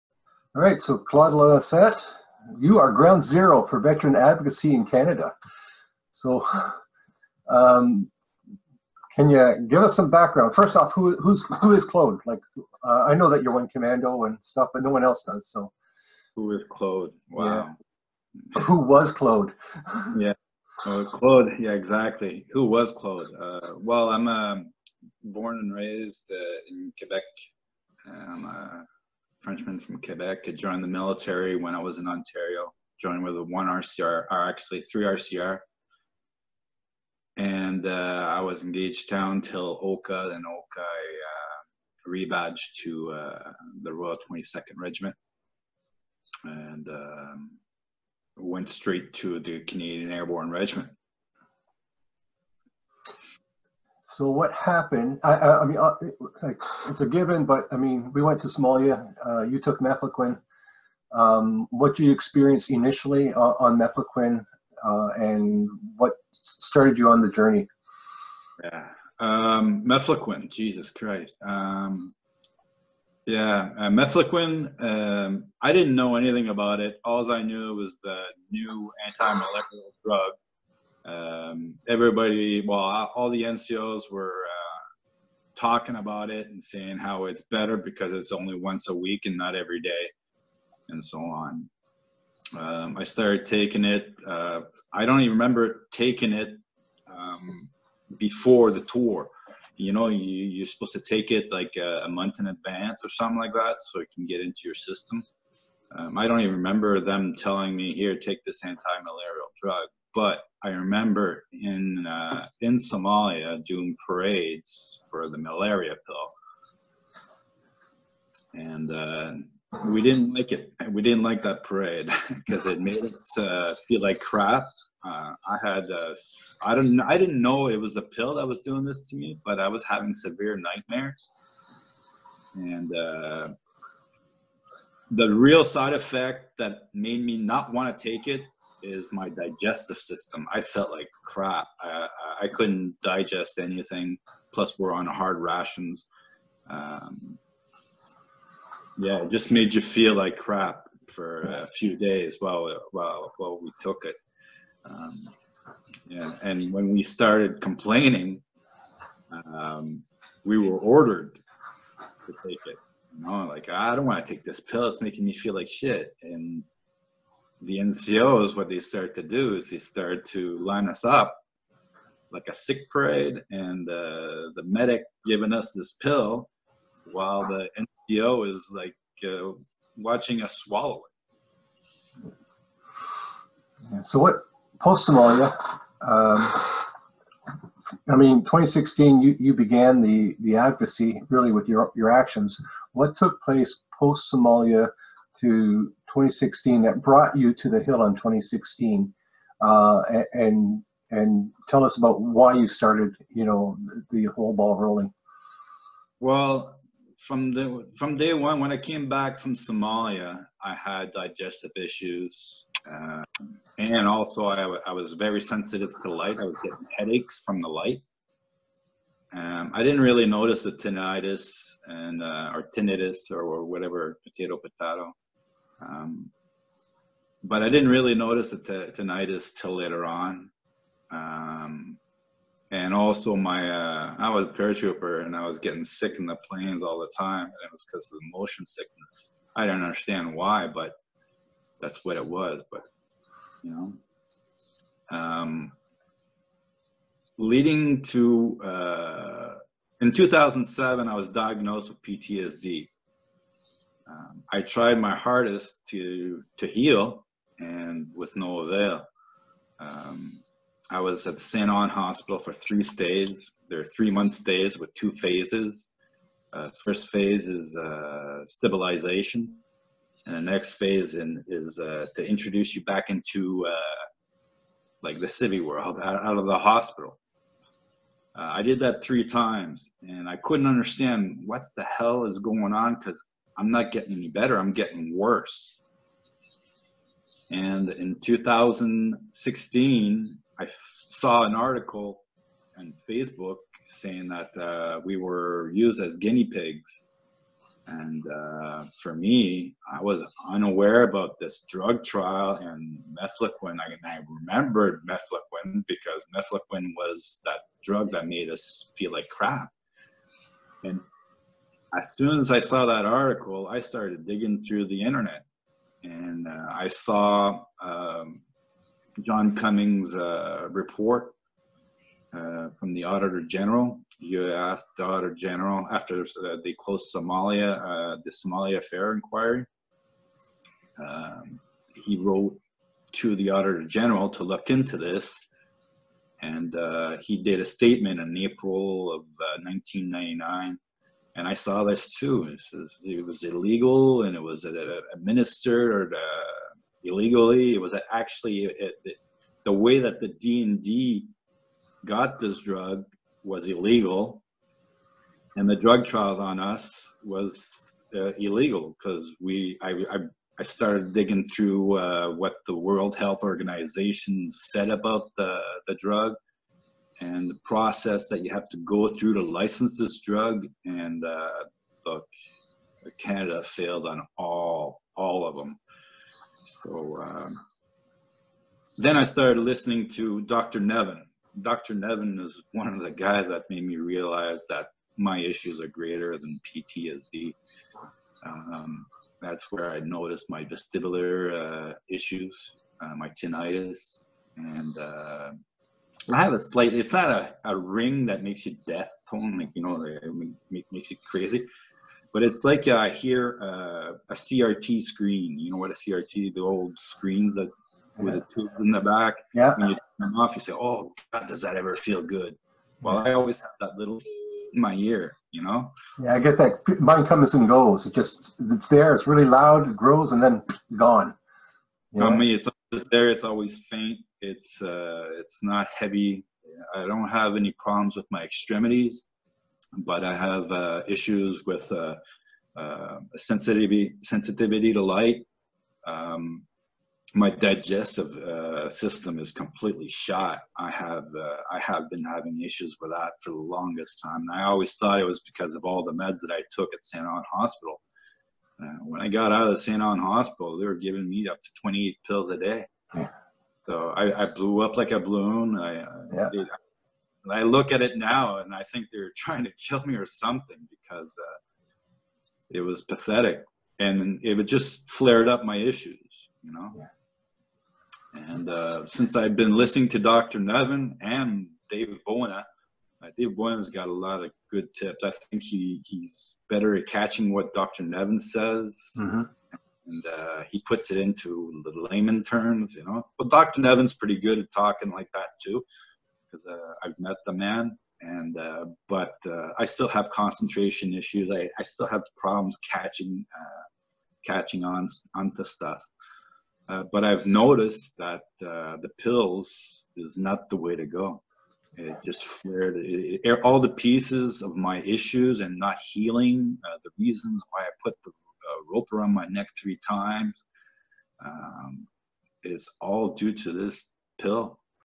Ground Zero Veterans Mefloquine Rally Part 1 of 4
With Claude Lalancette and Stephen Beardwood

Claude's Hunger Strike on Sept 19 2016 was the catalyst that began the annual Veterans Mefloquine Rally on Parliament Hill. You will find Veterans Protesting on Sept 19 every year until the Government of Canada acknowledges the damage of Mefloquine.